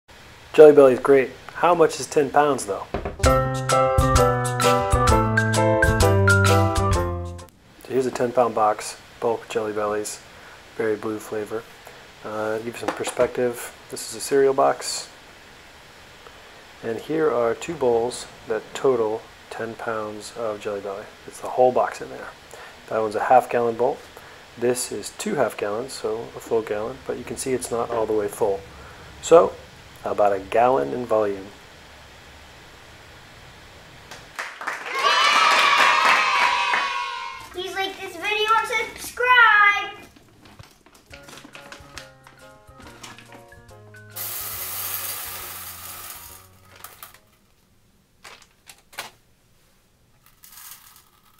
How Much is 10 Pounds of Jelly Bellys? - CandyStore.com
Our How Much Candy series features different bulk candy products to show you the most basic question everyone has:  How much is that?

Everyone loves Jelly Belly, but you're used to buying it in theater size boxes or gift box size assortments.  You've done the flavor testing, and been bean-boozled.  Now you're ready to commit to 10 pounds of bulk Jelly Bellys.  But how much is that?

How many bowls or jars or vases or fish ponds or swimming pools can I fill up with 10 pounds?  We break it down in the video above. Here's the numeros si tu prefieras:

10 pounds of Jelly Bellys = a little more than 1 gallon in volume

Gallon of milk vs. gallon of Jelly Bellys... which one would last longer in your house?